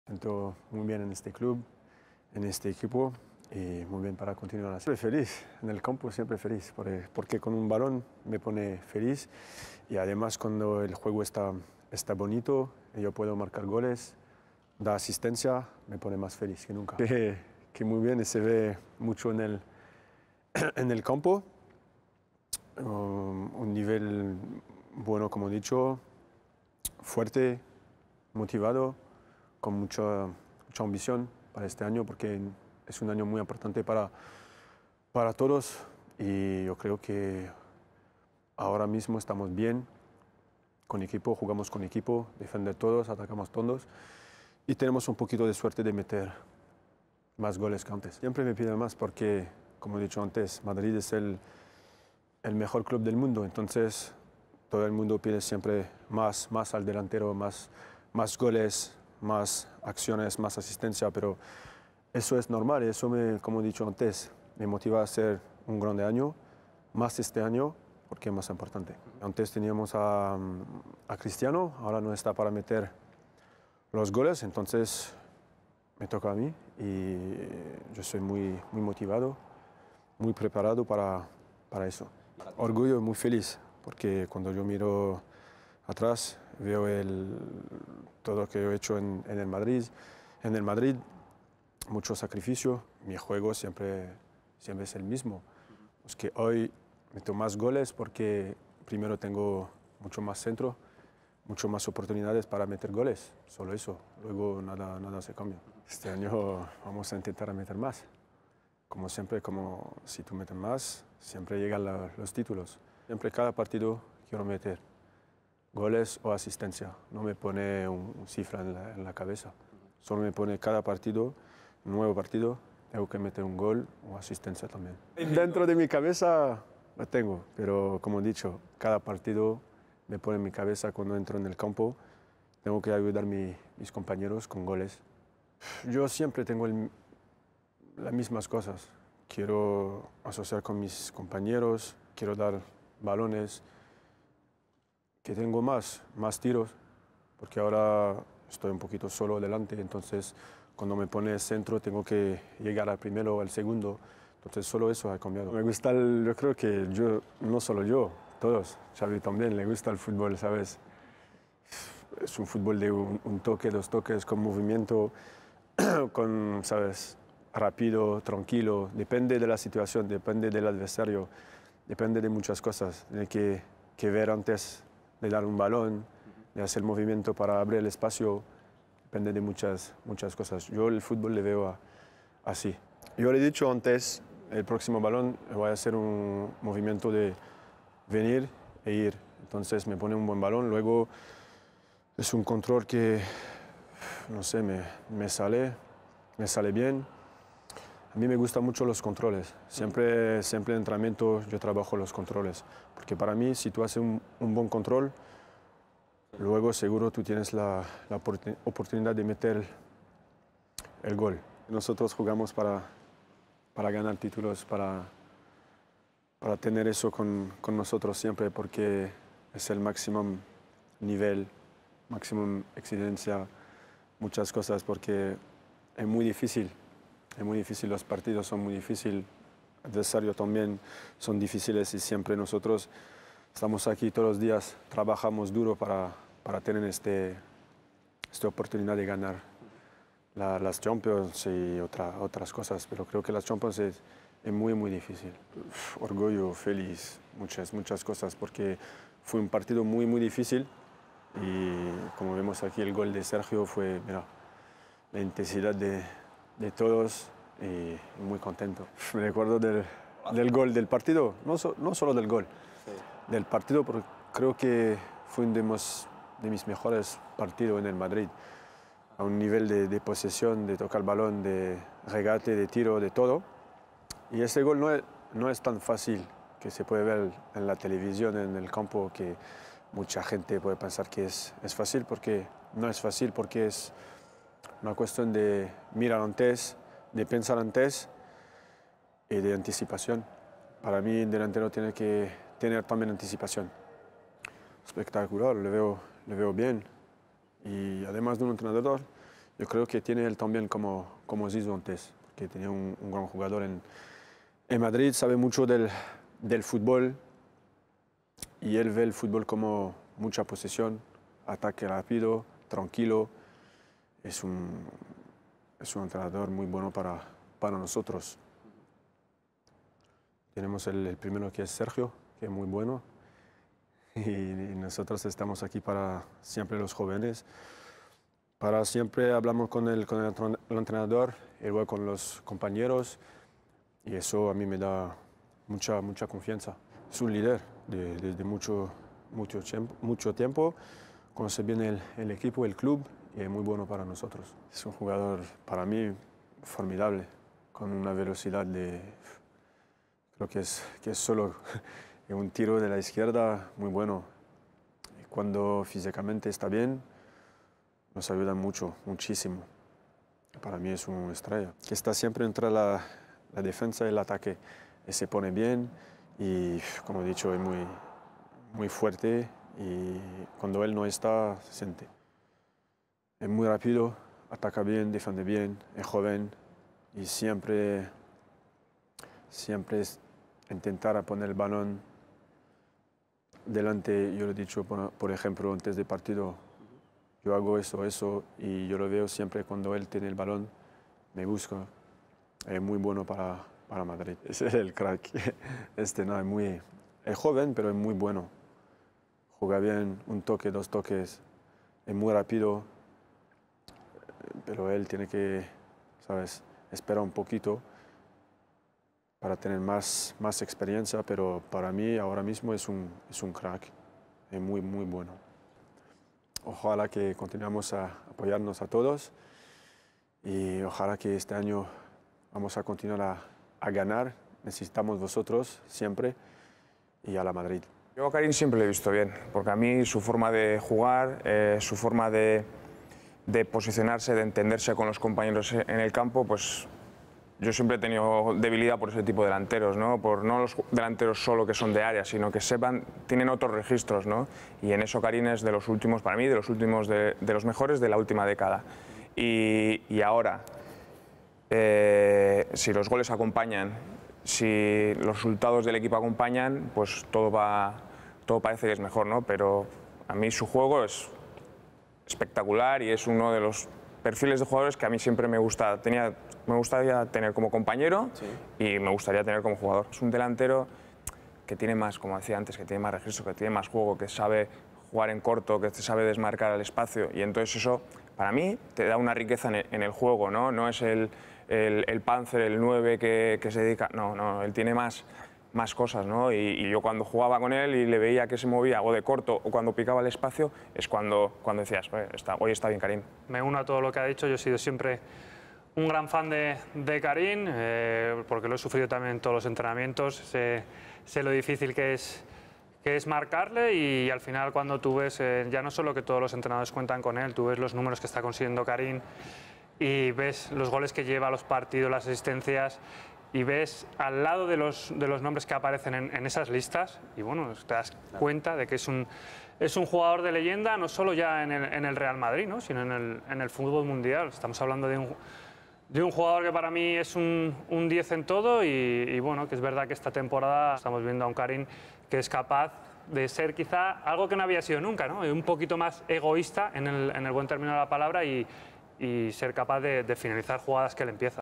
BENZEMA: "Real Madrid is the best club in the world." | EXCLUSIVE INTERVIEW
Enjoy our exclusive interview with Real Madrid's No.9 Karim Benzema, who discussed a range of topics on Realmadrid TV, including his thoughts on Sergio Ramos, Gareth Bale, Vinicius Junior and more! The interview features appearances by club legends Xabi Alonso and Álvaro Arbeloa!